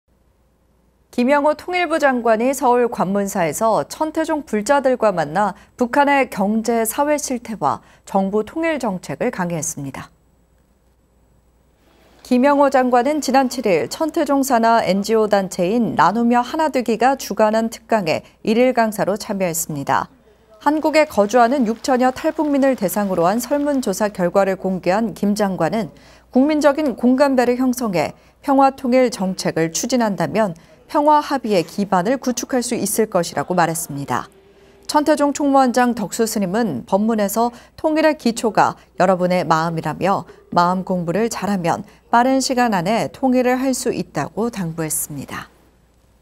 [BTN뉴스] 김영호 통일부 장관, 관문사서 남북 통일정책 강연
김영호 통일부장관이 서울 관문사에서 천태종 불자들과 만나 북한의 경제·사회 실태와 정부 통일정책을 강의했습니다.

김영호 장관은 지난 7일 천태종 산하 NGO단체인 나누며하나되기가 주관한 특강에 일일강사로 참여했습니다.

한국에 거주하는 6000여 탈북민을 대상으로 한 설문조사 결과를 공개한 김 장관은 “국민적인 공감대를 형성해 평화통일 정책을 추진한다면 평화 합의의 기반을 구축할 수 있을 것”이라고 말했습니다.

천태종 총무원장 덕수스님은 법문에서 “통일의 기초가 여러분의 마음”이라며 “마음공부를 잘하면 빠른 시간 안에 통일을 할 수 있다”고 당부했습니다.